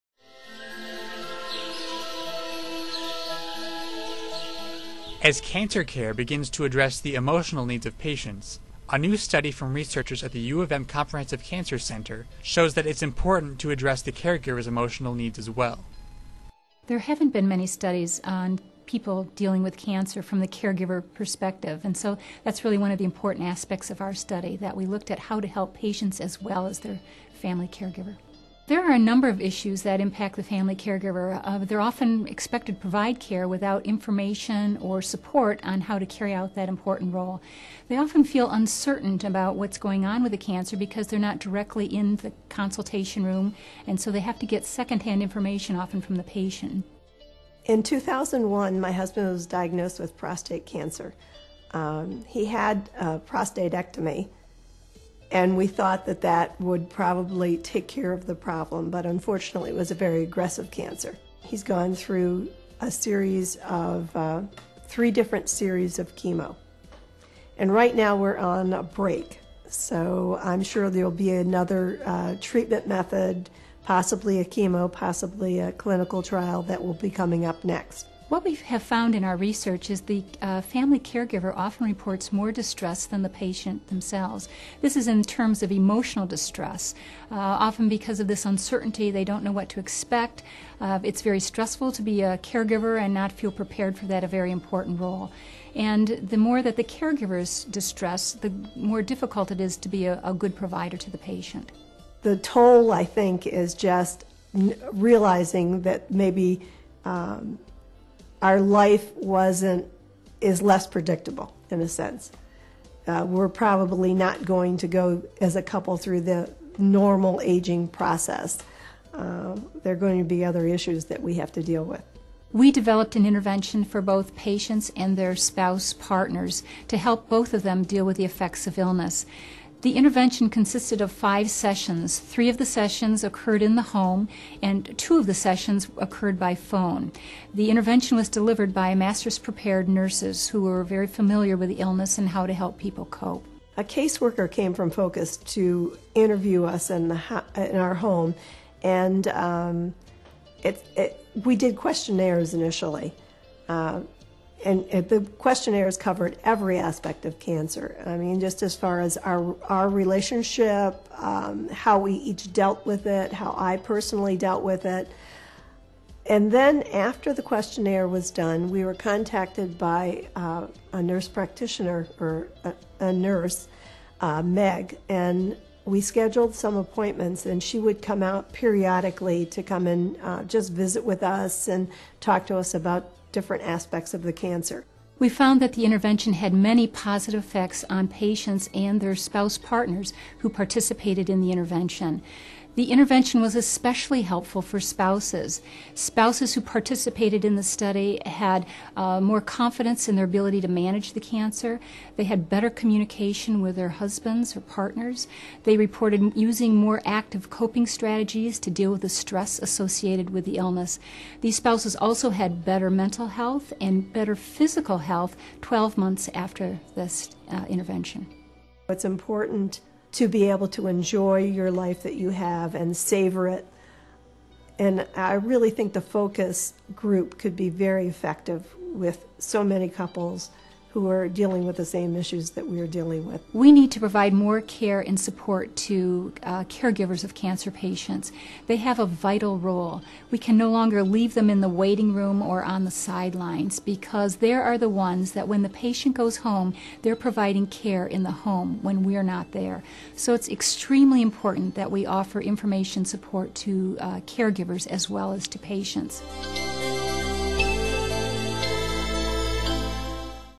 Caregivers benefit from cancer support programs, U-M study f
As cancer care begins to address the emotional needs of patients, a new study from researchers at the University of Michigan Comprehensive Cancer Center shows that it's important to address the caregiver's emotional needs as well.